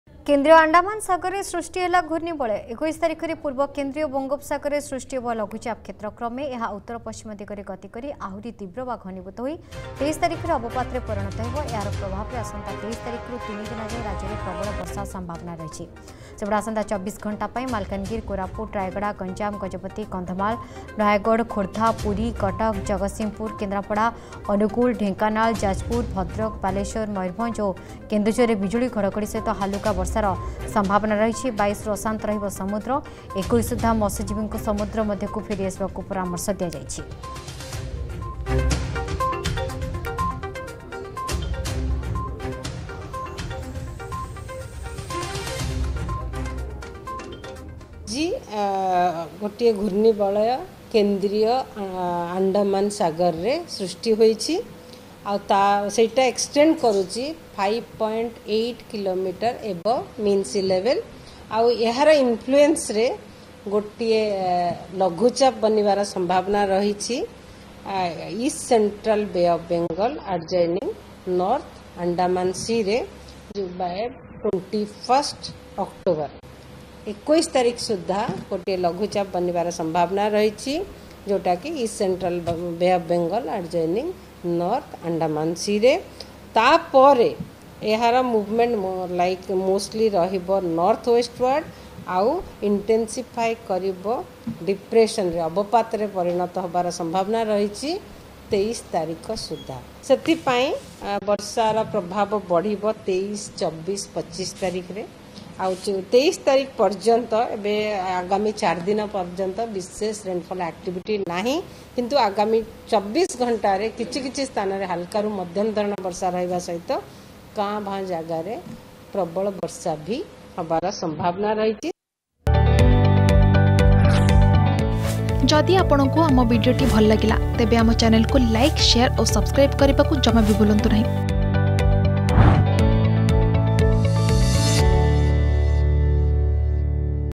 ଓଡ଼ିଶାକୁ ବାତ୍ୟା ଭୟ ! | Cyclone Update
କେନ୍ଦ୍ରୀୟ ଆଣ୍ଡାମାନ ସାଗରରେ ସୃଷ୍ଟି ହେଲା ଘୂର୍ଣ୍ଣିବଳୟ ।  ୨୧ ତାରିଖରେ ପୂର୍ବ କେନ୍ଦ୍ରୀୟ ବଙ୍ଗୋପସାରରେ ସୃଷ୍ଟି ହେବ ଲଘୁଚାପ କ୍ଷେତ୍ର । ୨୩ ତାରିଖରେ ଅବପାତରେ ପରିଣତ ହୋଇ ଆସନ୍ତା ୩ ଦିନ ଯାଏଁ ରାଜ୍ୟରେ ପ୍ରବଳ ବର୍ଷା ସମ୍ଭାବନା।